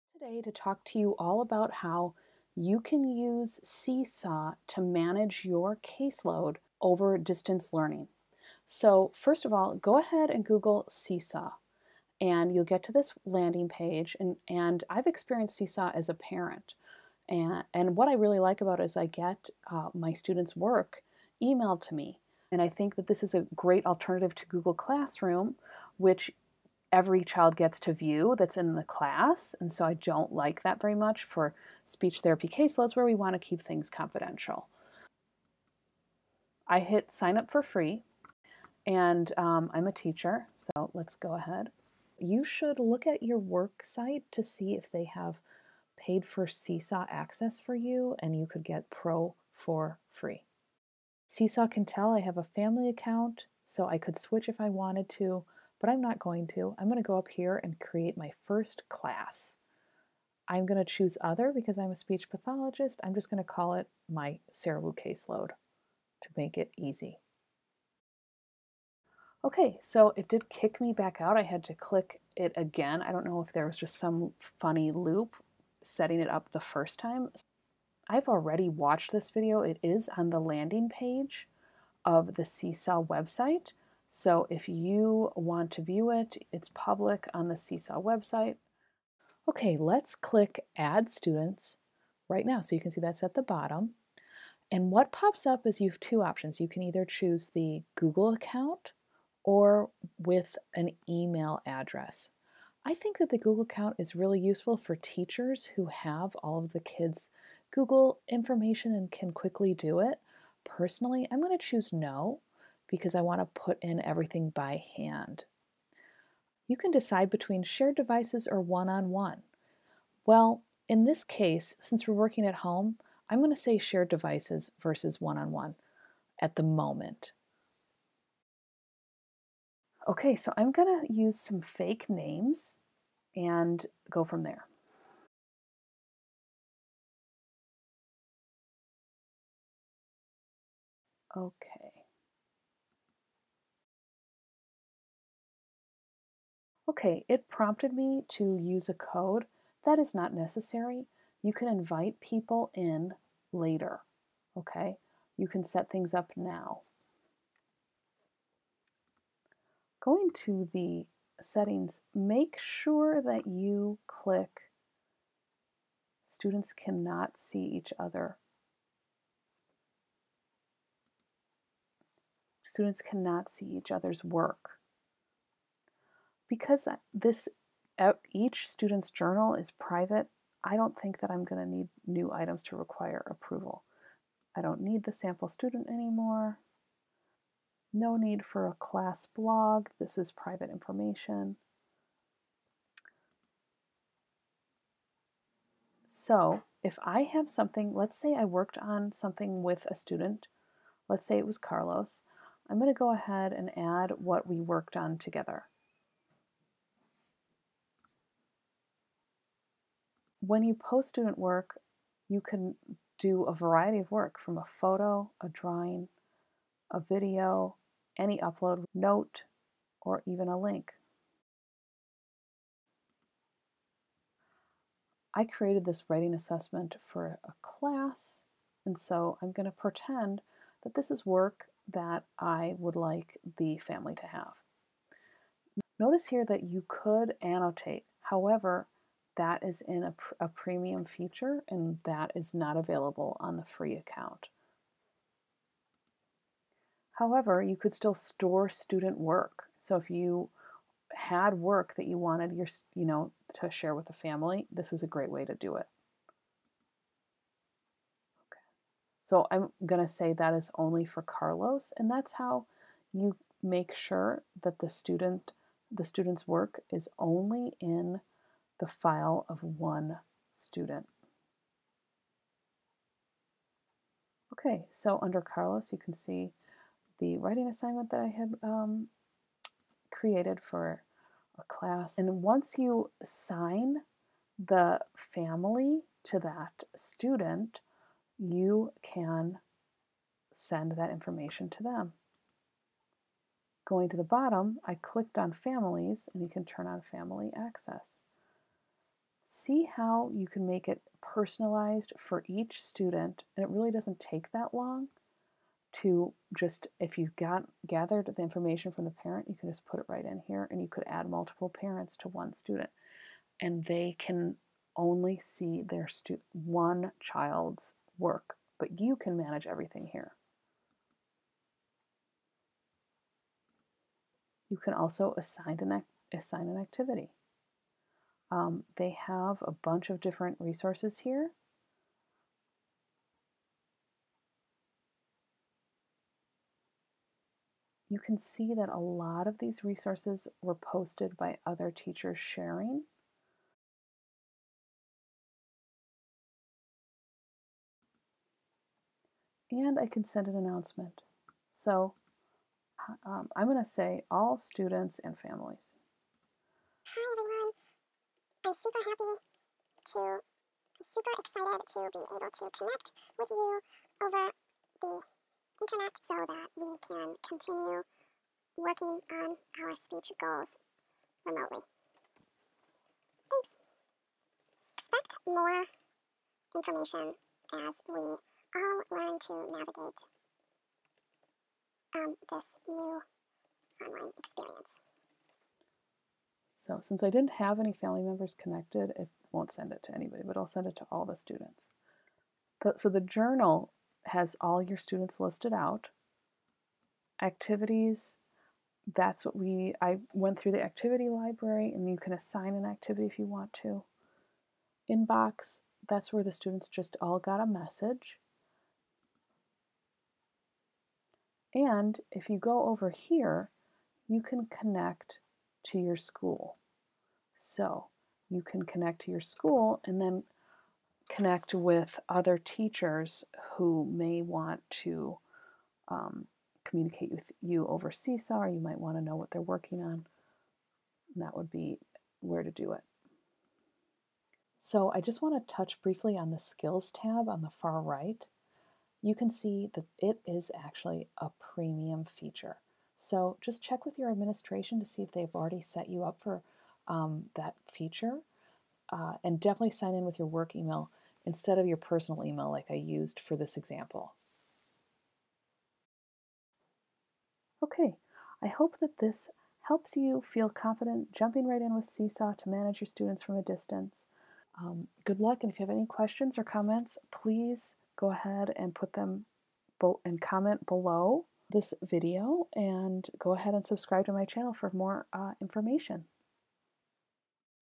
Seesaw Tutorial for Speech Pathologists to Communicate with Parents over Distance Learning
How are you managing your parent communication during distance learning? Seesaw might be the best thing around for speech paths to share student work or homework with parents in a secure and private way. I've heard that for Google Classroom you need to create a separate class for each student (because if you have one class, all the students can see everything). Seesaw is way better because just by changing one setting it is private: the student and the student's family are the only ones who can view the account. Watch the video Seesaw Tutorial for Speech Pathologists to learn what Seesaw is all about. I think this tool is valuable to use to communicate with parents even after distance learning ends.